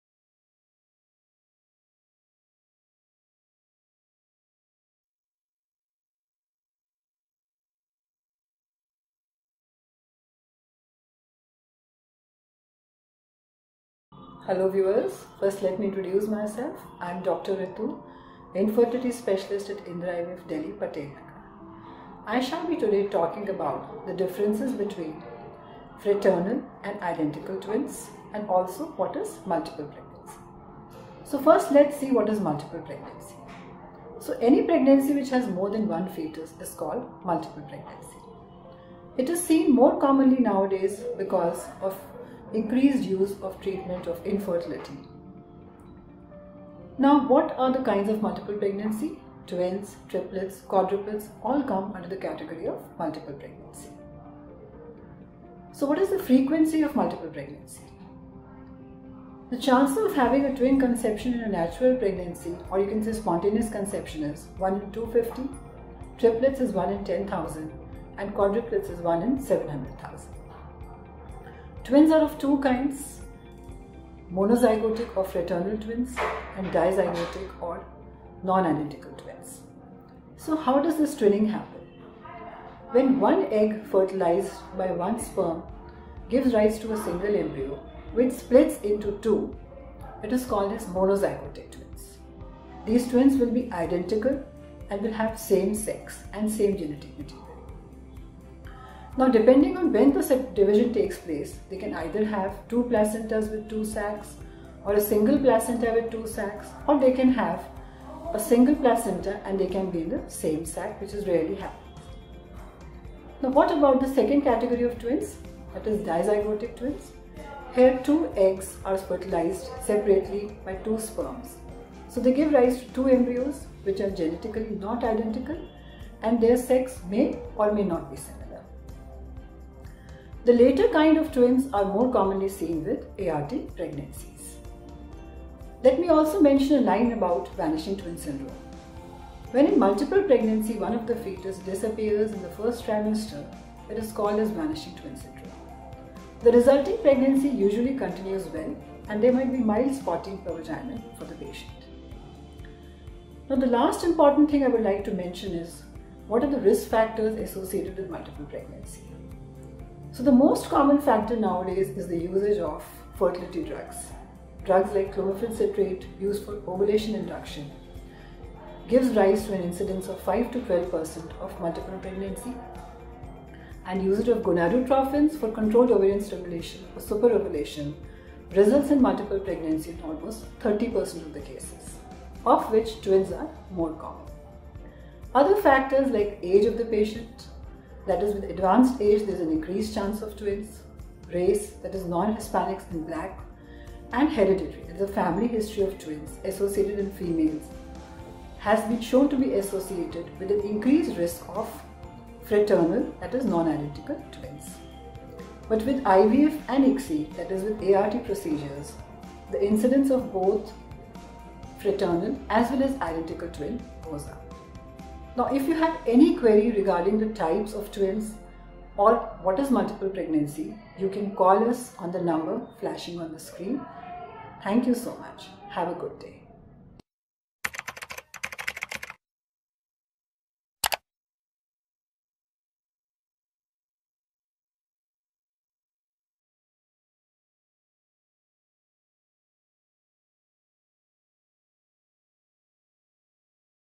3 Major Differences between Identical Twins & Fraternal Twins
✅ All about Multiple Pregnancy
✅ How can one tell if twins are identical or fraternal?
✅ Which is more common?

Know everything about identical twins & fraternal twins from Indira IVF’s leading fertility expert Dr. Ritu Punhani.

You can also talk to our experts directly: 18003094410